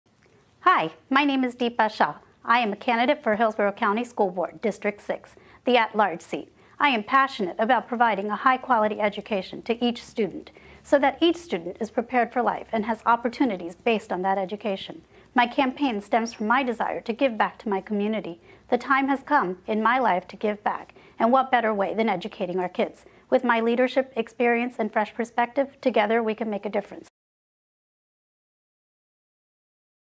Dipa Shah
30-second commercial for Valrico FishHawk Chamber of Commerce member Dipa Shah